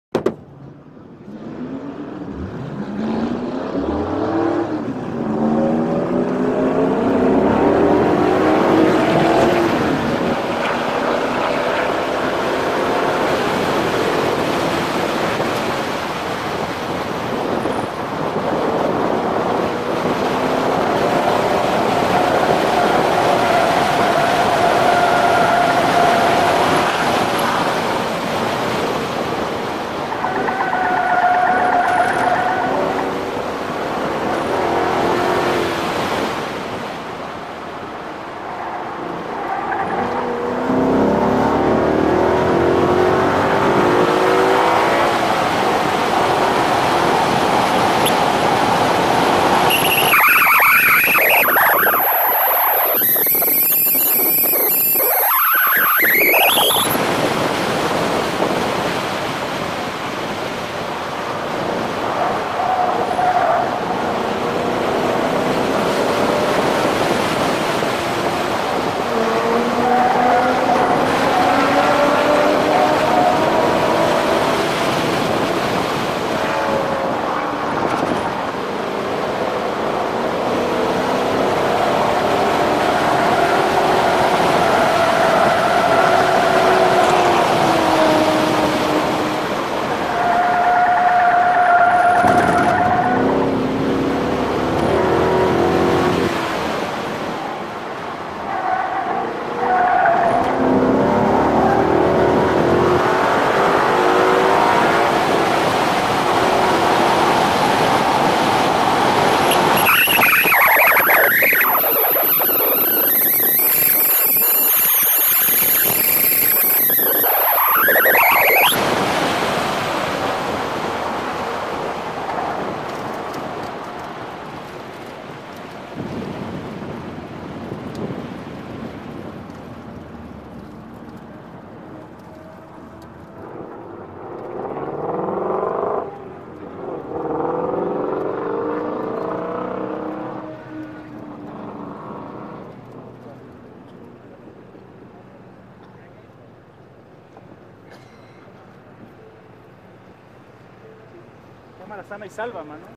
Dragon Gonzales 360G ( Mustang Shelby 350 GT)
Este fue un video grabado en el Autodromo de Amozoc durante el evento de Ford performance 2016